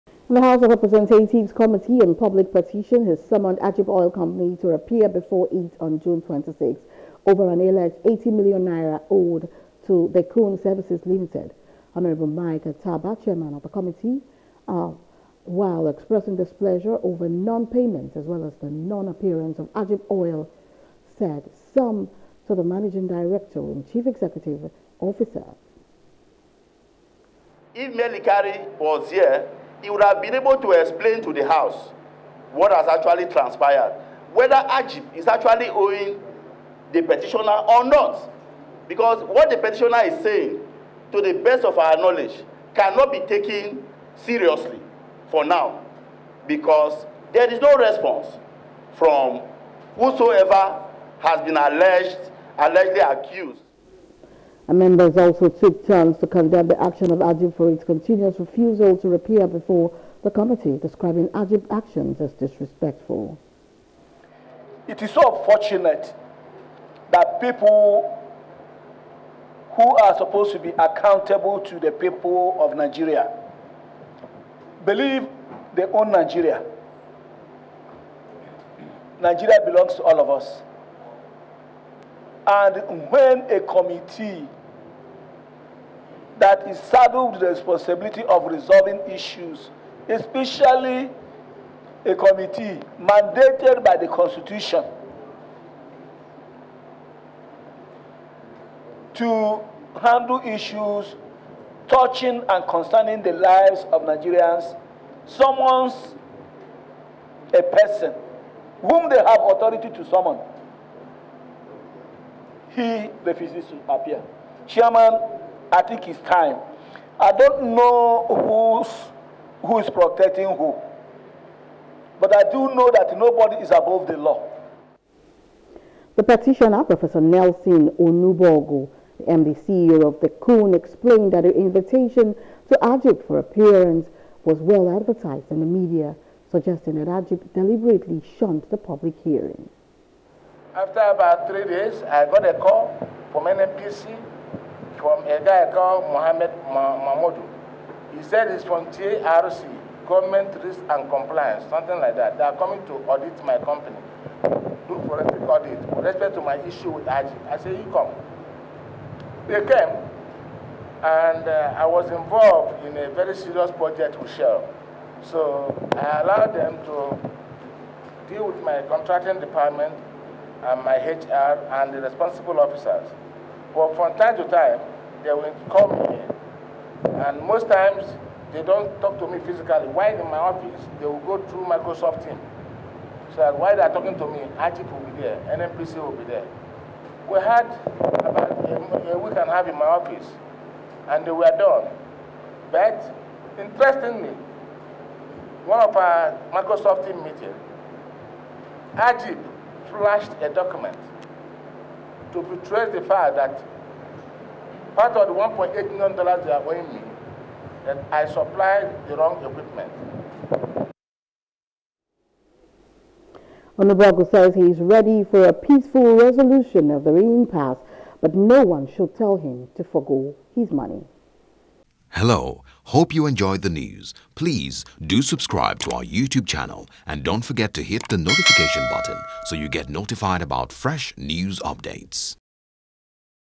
Alleged $80m Debt: Reps. Public Petition Committee Summons Agip Oil
The House of Representatives Committee on Public Petition has summoned AGIP Oil Company to appear before it on June 26 over an alleged N80 million owed to De Coon Services Ltd.